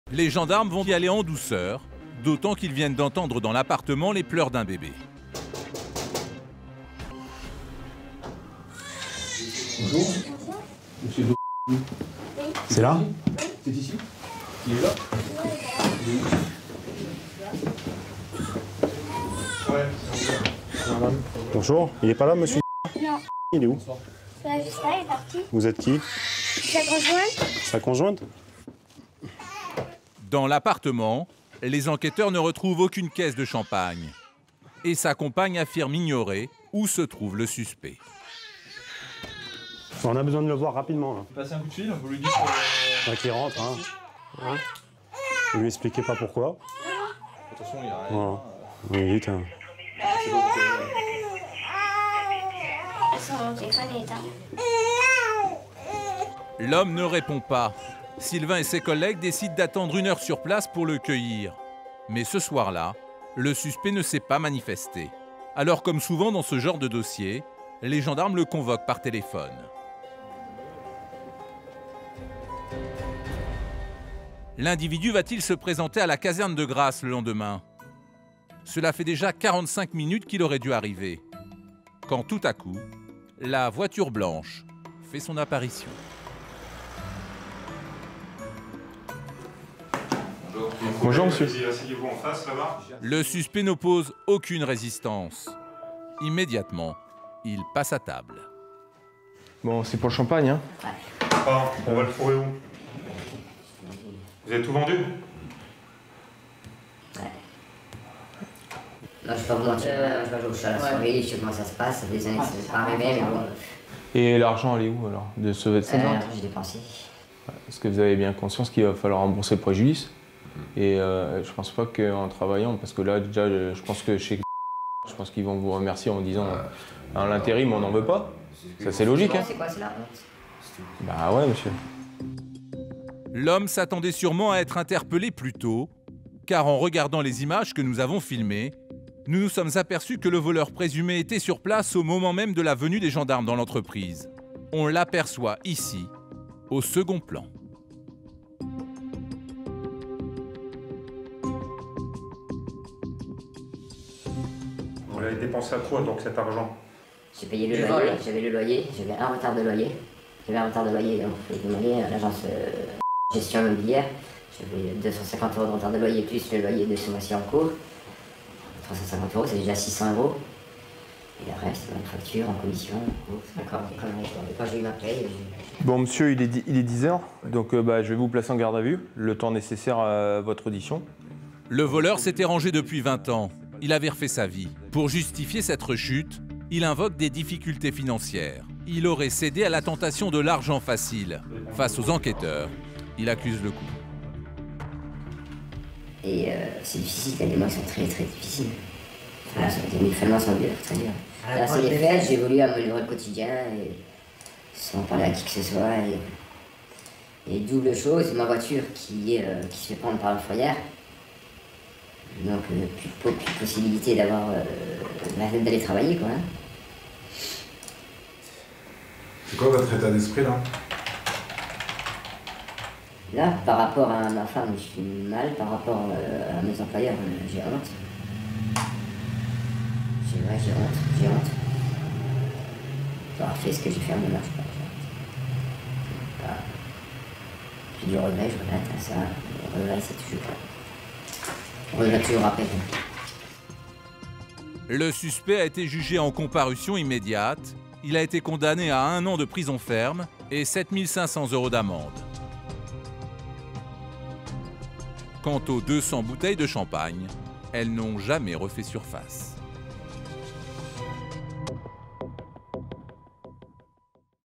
20 ans sans commettre un délit et c’est la boulette
La gendarmerie réussit à mettre la main sur le voleur de champagne